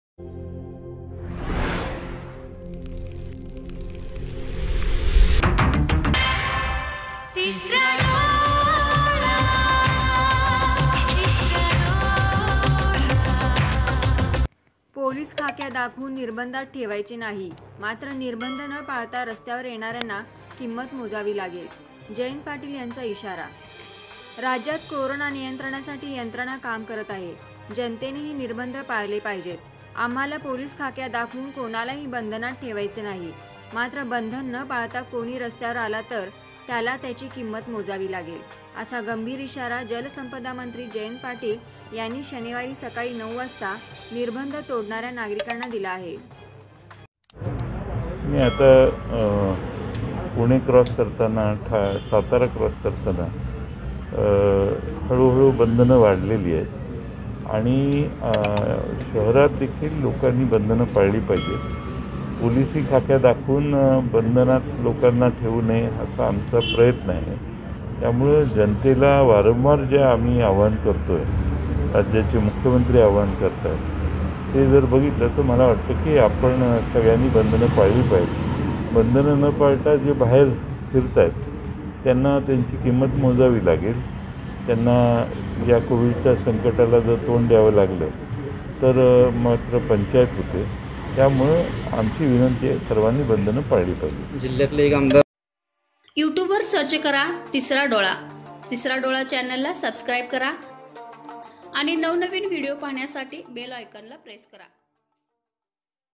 अन्यथा किंमत मोजावी लागेल पालकमंत्र्यांचा इशारा
पोलिसी खाक्या दाखवून निर्बंधात ठेवायचं नाही ; मात्र निर्बंध न पाळता रस्त्यावर येणाऱ्यांना किंमत मोजावी लागेल ; जयंत पाटील यांचा इशारा